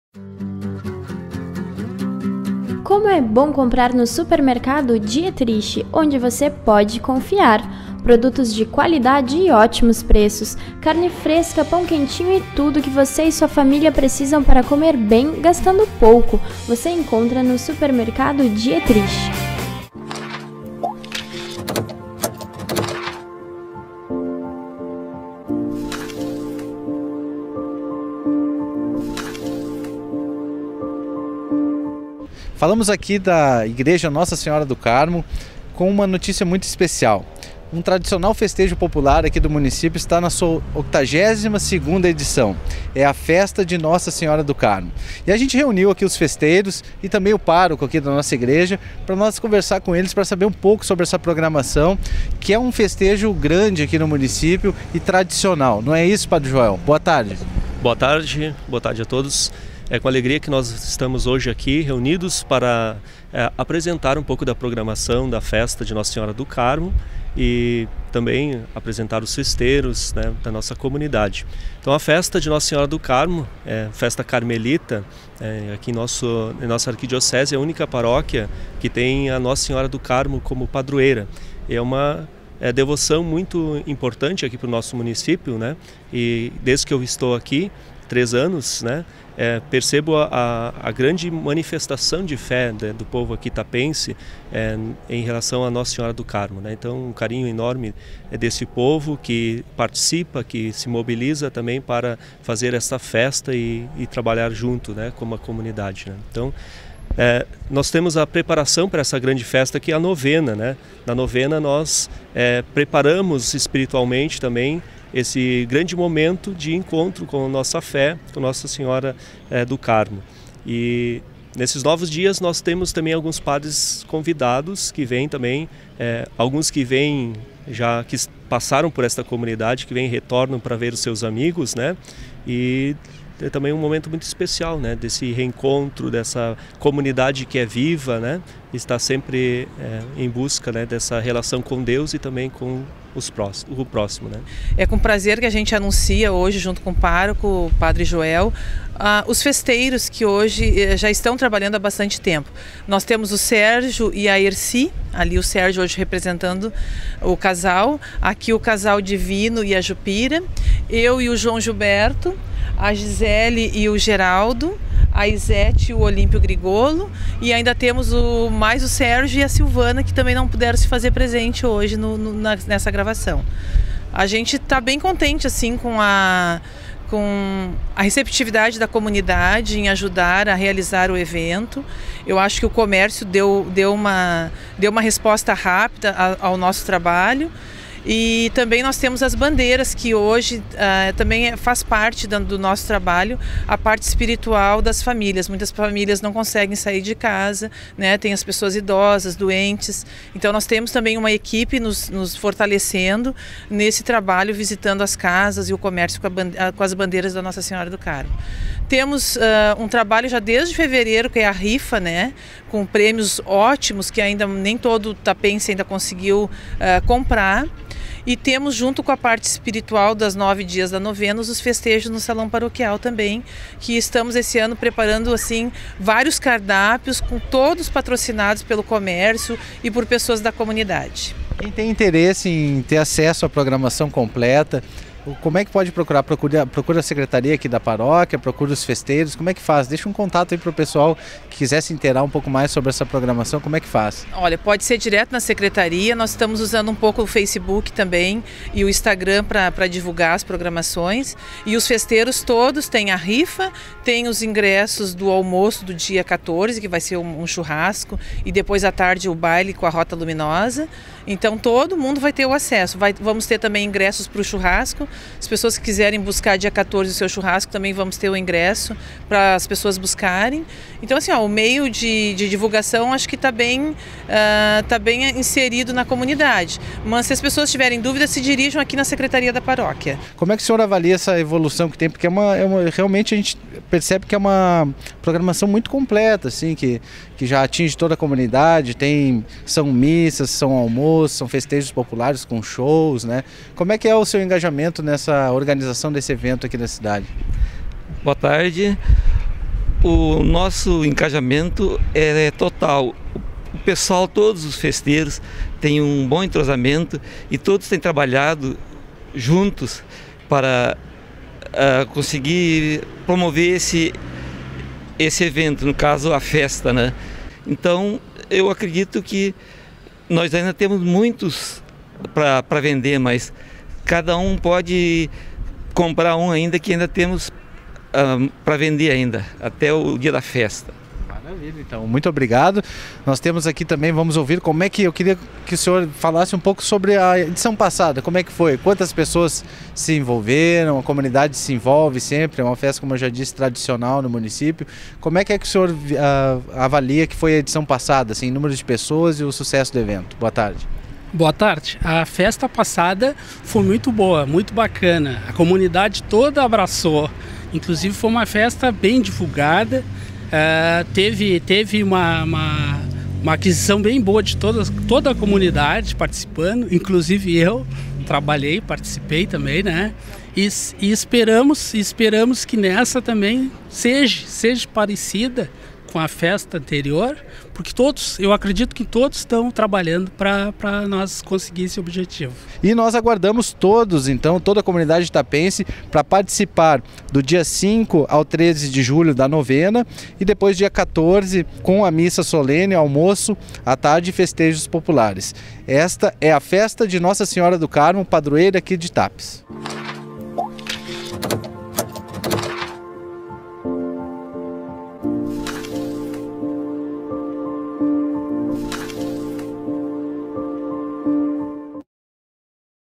Festeiros convidam comunidade para a festa de Nossa Senhora do Carmo, padroeira de Tapes.
A programação religiosa e social da 82ª Festa de Nossa Senhora do Carmo contempla diversas atividades de 5 a 14 de julho. Confira em detalhes como será a edição 2019 da importante festa popular que reúne centenas de fiéis de Tapes e região todos os anos.